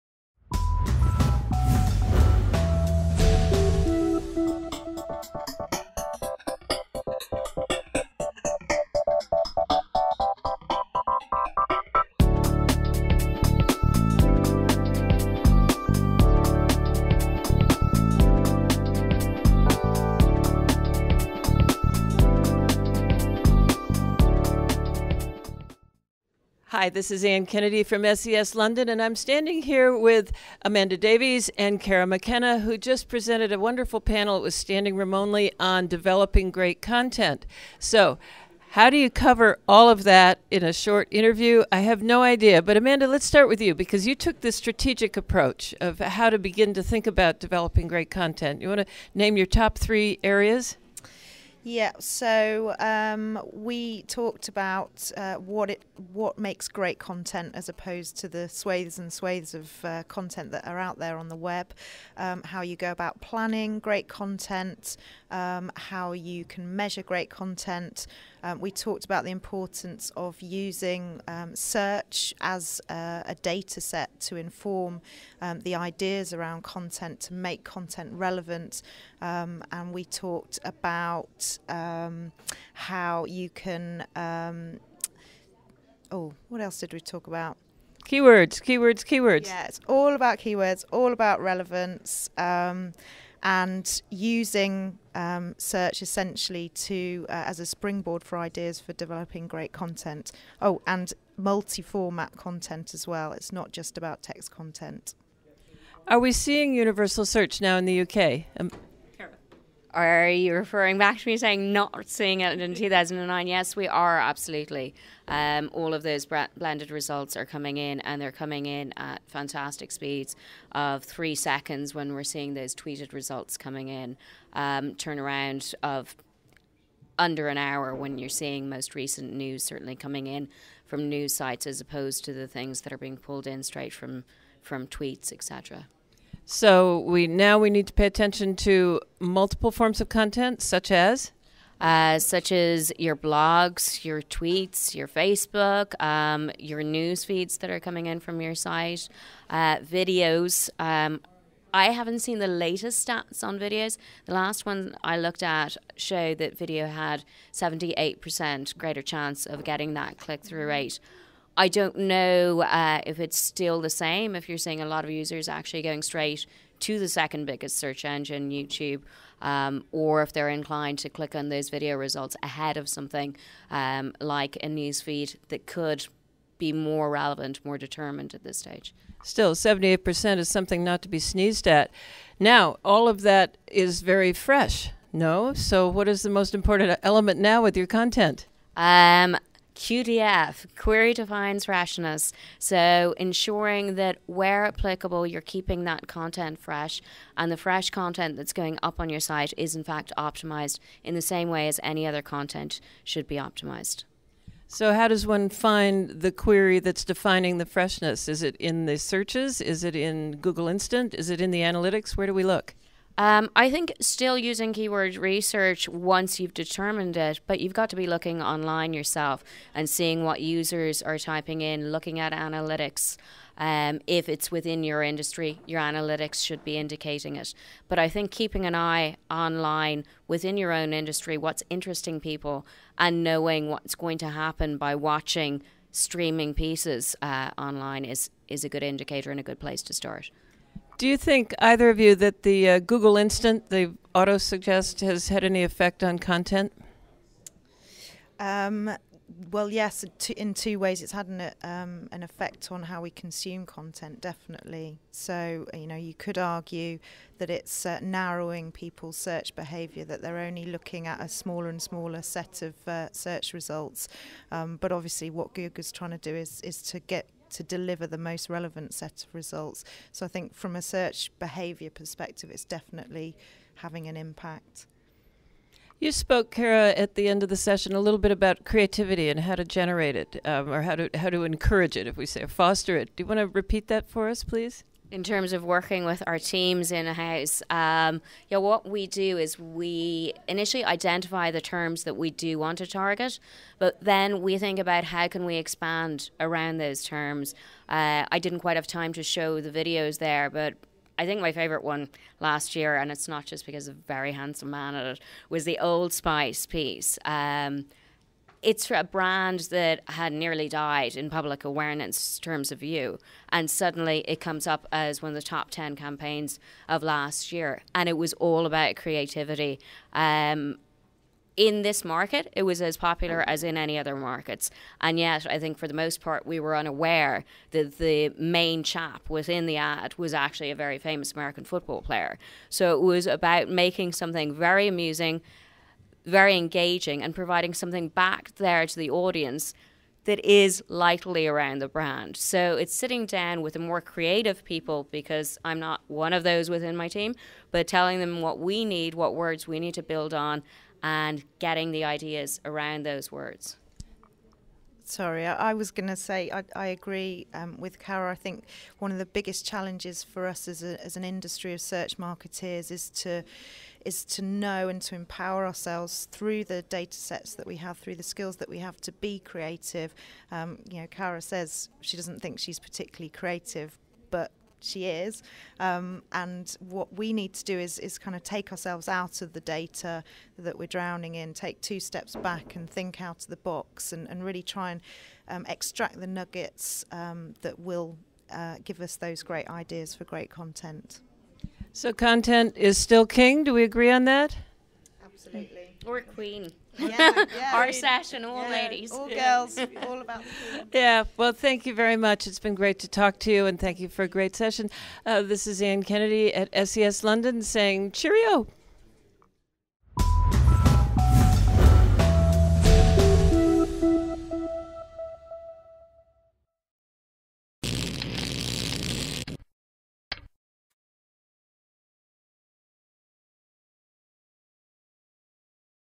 Creating great online, fresh content SES London 2011
Anne Kennedy, Beyond Ink, interviews Amanda Davie, founder and managing director, Reform, and Caragh McKenna, senior manager, seo, The Search Agency, on the subject of developing great content at SES London 2011. Amanda says using search as a data set to inform the ideas around content and make it relevant is important as well as using multiformat content and essential keywords. Caragh says universal search is being used in the UK and blended results are coming in at fantastic speeds of three seconds or less.  Caragh says marketers must pay attention to content sources coming from blogs, twitter, facebook, newsfeeds, and videos. Caragh says where applicable, marketers must keep updating their content so it is fresh and optimized at all times.  Amanda says Google instant (auto-suggest) has impacted the way users consume content and its narrowing people's search behavior, looking at a smaller set of search results. Caragh says to foster creativity, you must do the following: identify you're the key terms you want to target, and figure out how to expand around those terms, integrating the long-tail.